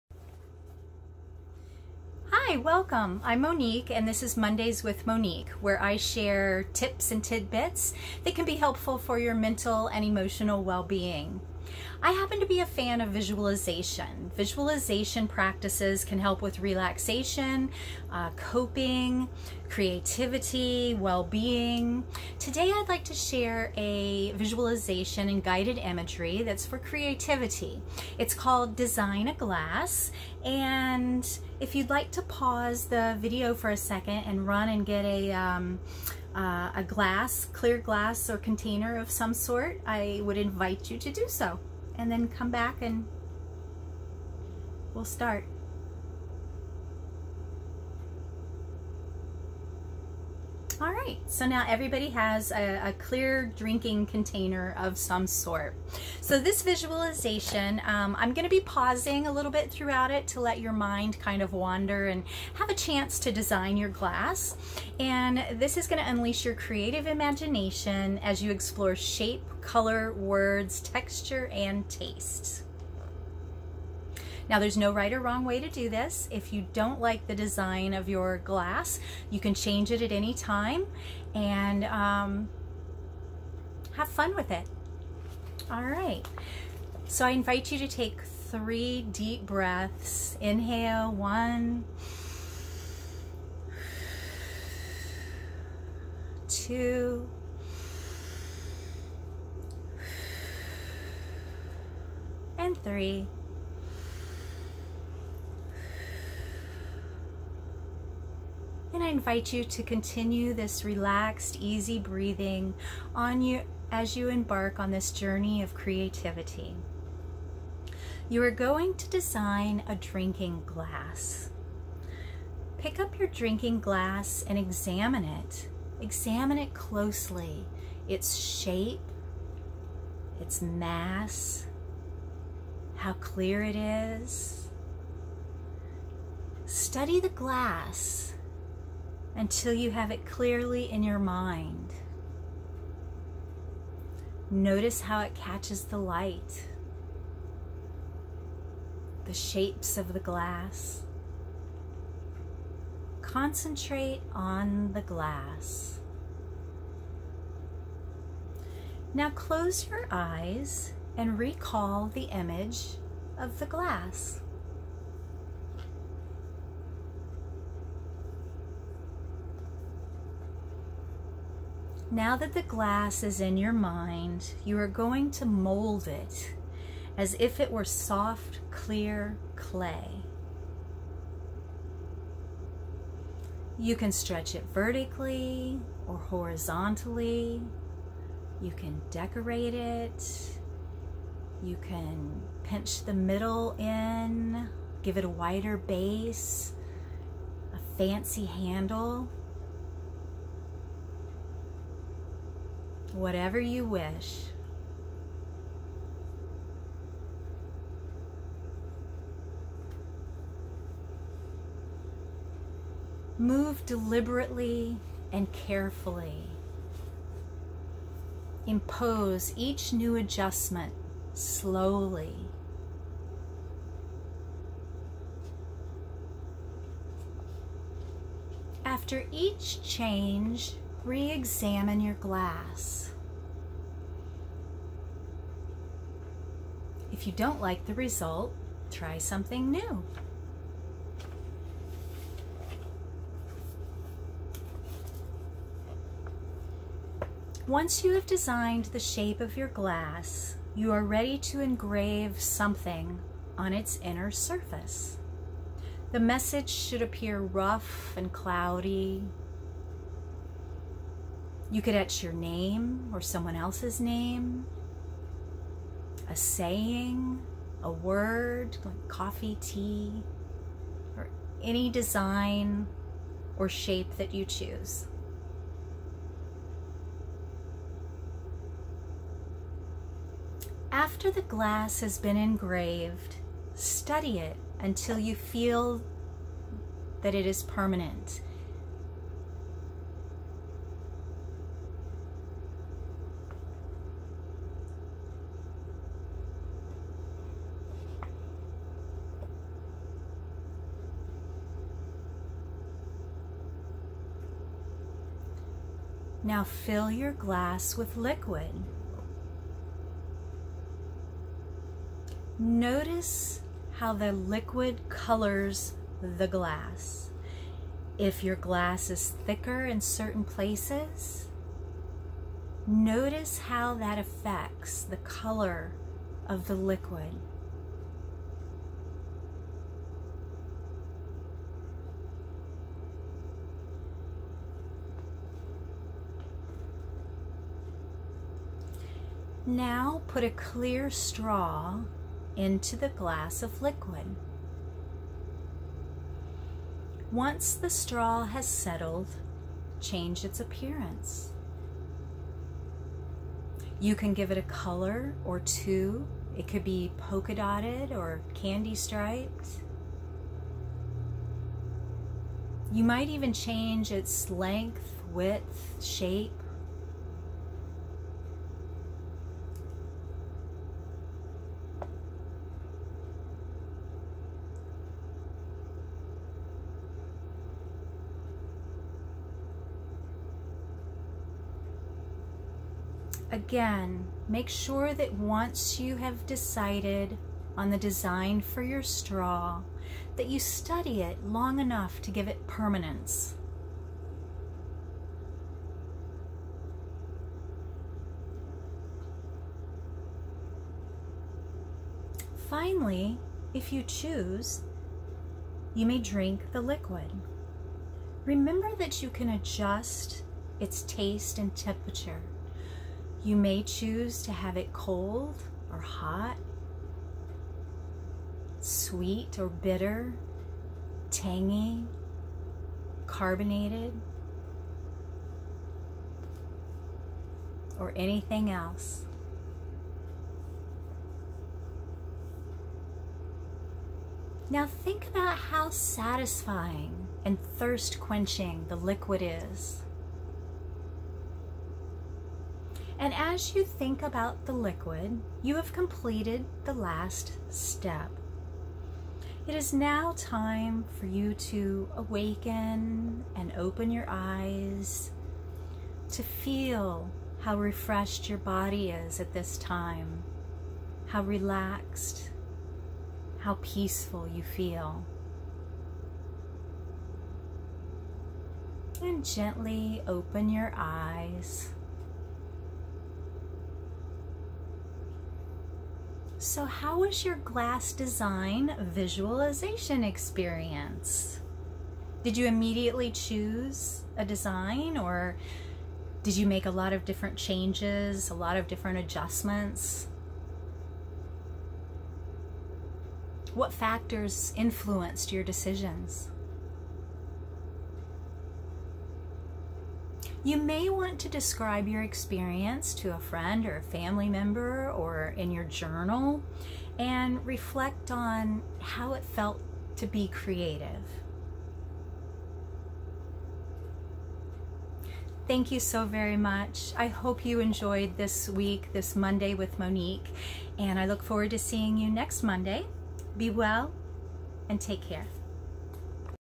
Mondays with Monique
Welcome to Mondays with Monique featuring Monique Heckler, MSW.

Monique is a therapist in the Fort Myers office and shares advice and tips that can help overall mental wellbeing.

In today's episode, Monique discusses visualization, to enhance creativity through sensory images (or guided imagery). Explore your ability to visualize.

This is appropriate for anyone exploring the role of creativity in problem-solving and self-fulfillment.